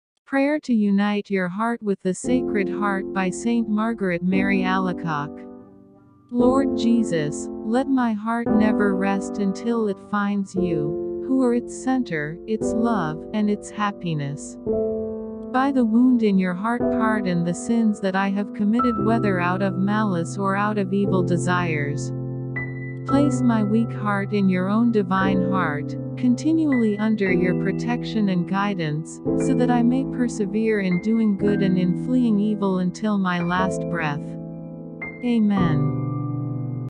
A Prayer to unite your heart with the Sacred Heart by ST. MARGARET MARY ALACOQUE ||Feast Day: Oct 16
Lord Jesus, let my heart never rest until it finds You, who are its center, its love, and its happiness.
By the wound in Your heart
pardon the sins that I have committed
whether out of malice or out of evil desires.

Place my weak heart in your own divine heart,continually under your protection and guidance,so that I may persevere in doing good and in fleeing evil until my last breath.
Amen.

🌹 MORE ABOUT OF ST. MARGARET MARY ALACOQUE🌹

St Margaret Mary Alacoque, a French Roman Catholic Visitation nun and mystic, is greatly recognized for her devotion to the Sacred Heart of Jesus.

She was born in 1647 in France as the only daughter of Claude and Philiberte Lamyn Alacoque. Margaret has always shown an intense love for the Blessed Sacrament and preferred silence over typical childhood play.

She continued this until rheumatic fever confined her to her bed for four years. After making a vow to the Blessed Virgin Mary to consecrate herself to religious life, Margaret instantly returned to perfect health.

Margaret Mary died a couple of years later, at the age of 43, on October 17, 1690, while being anointed. She spoke the words, "I need nothing but God, and to lose myself in the heart of Jesus."

St Margaret Mary Alacoque was officially canonized on May 13, 1920 by Pope Benedict XV and, in 1928, Pope Pius XI upheld the Church's position regarding the credibility behind her visions of Jesus Christ. He stated Jesus "manifested Himself" to Margaret and the chief features of devotion to the Sacred Heart are "reception of Holy Communion on the first Friday of each month, Eucharistic adoration during a 'Holy hour' on Thursdays, and the celebration of the Feast of the Sacred Heart."

St Margaret Mary Alacoque is the patron saint of devotees of the Sacred Heart, and those suffering with polio and from the loss of parents. Her feast day is celebrated on October 16. St Margaret Mary Alacoque, a French Roman Catholic Visitation nun and mystic, is greatly recognized for her devotion to the Sacred Heart of Jesus.

Hello everyone! I hope everyone is feeling great. The FEAST OF ST. MARGARET MARY ALACOQUE is on October 16.

❣❣❣❣ Credits / Sources ❣❣❣❣

John 11:40